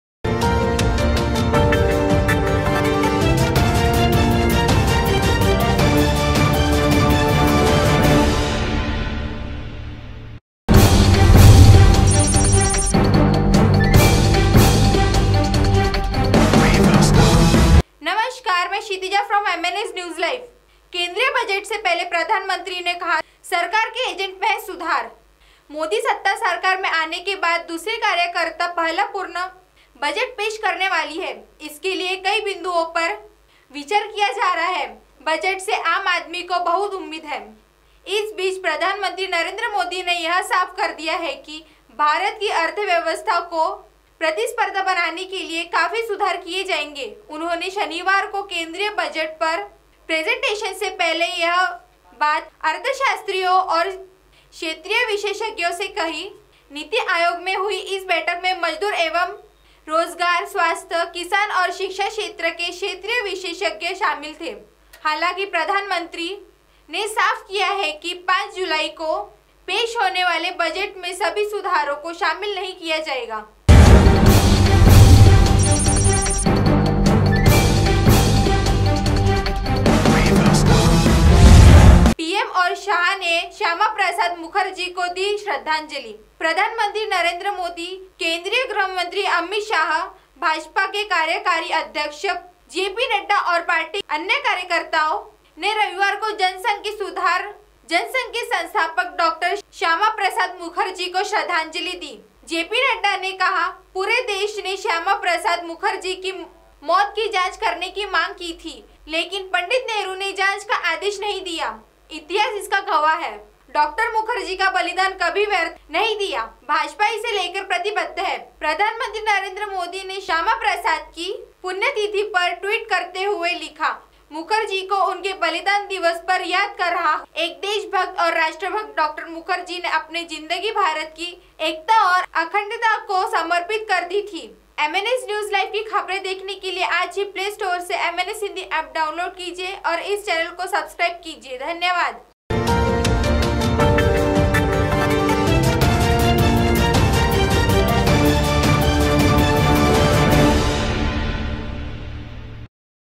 माेदी ने कहा सरकार के अजेंडे मे है सुधार
MNSNEWSLIVE पीएम माेदी ने कहा सरकार के अजेंडे मे है सुधार